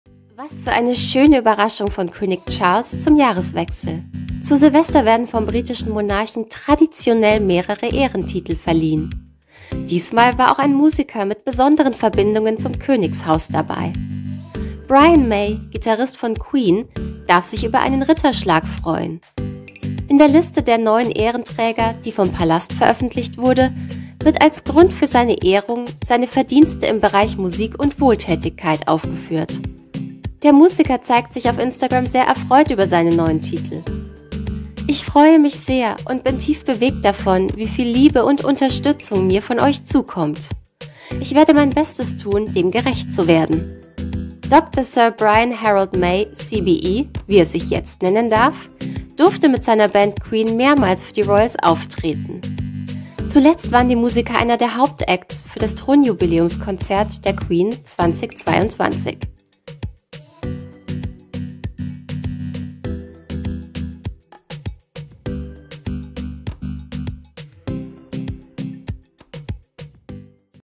König Charles trifft Queen: Besondere Ehre für IHN • PROMIPOOL
Was für eine schöne Überraschung von König Charles zum Jahreswechsel! Zu Silvester werden vom britischen Monarchen traditionell mehrere Ehrentitel verliehen.